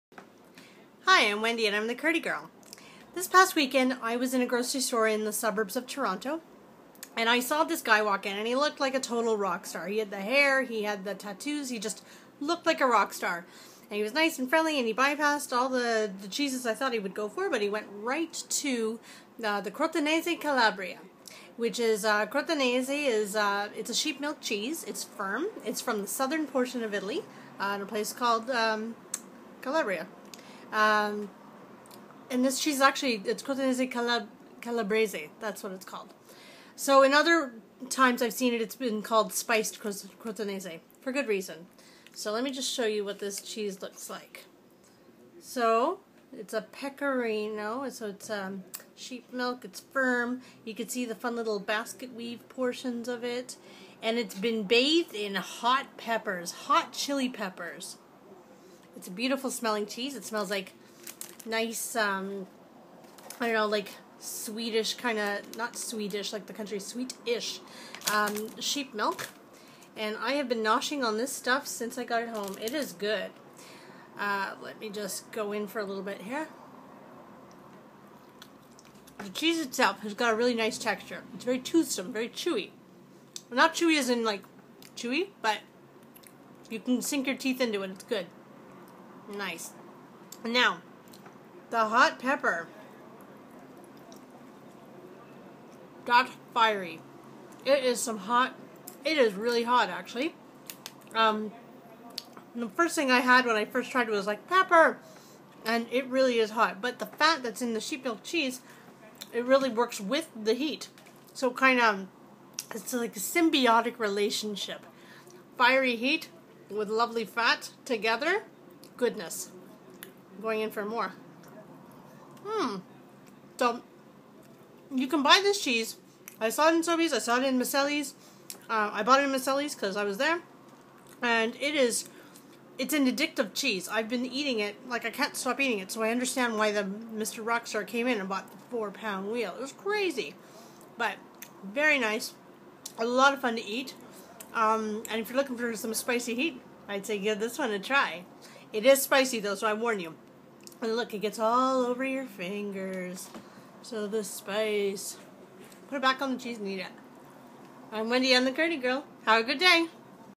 Crotonese Calabrese: a fiery treat!
A cheese bathed in hot chilis? Bring it on! Get the scoop on this fiery Italian treat, inspired by the shopping habits of a local rock star.